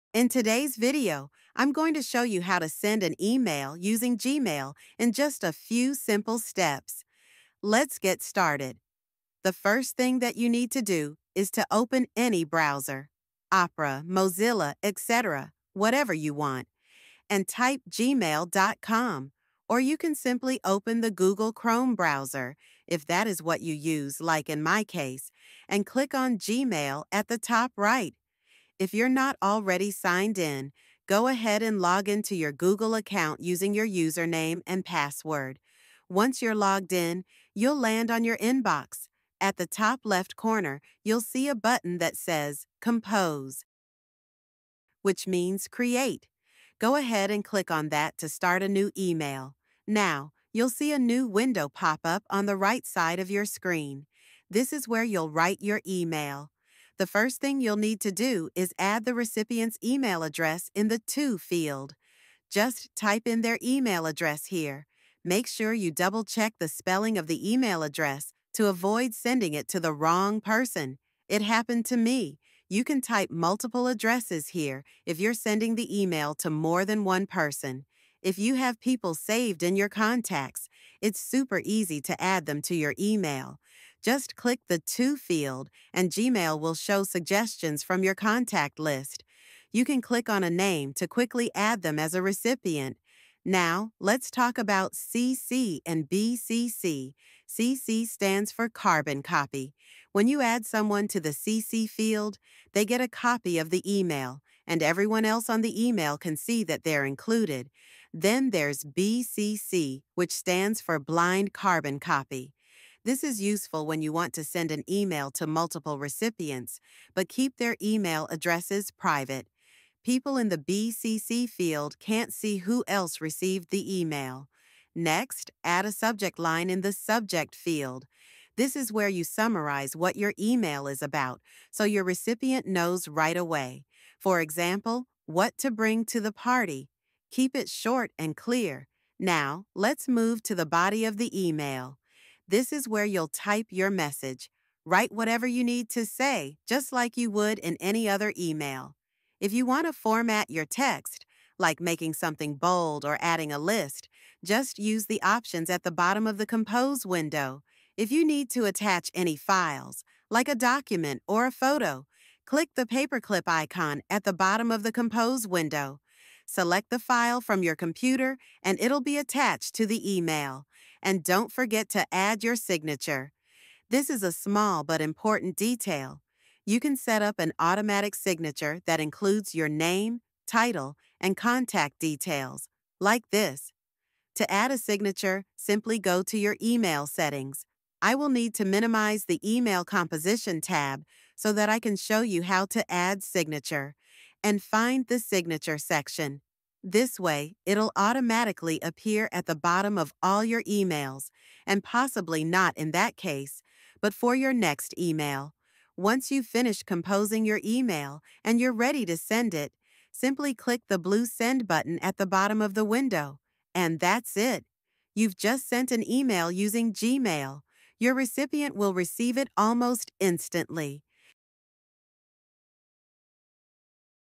How to Send an Email on Gmail Easy and Quick - Guide for Beginners 🥸
Learn how to send an email on Gmail in this easy-to-follow, step-by-step tutorial! Whether you're a beginner or just need a refresher, this video will guide you through the process of composing and sending emails using your Gmail account. From creating a new email to adding attachments and using advanced features, we’ve got you covered!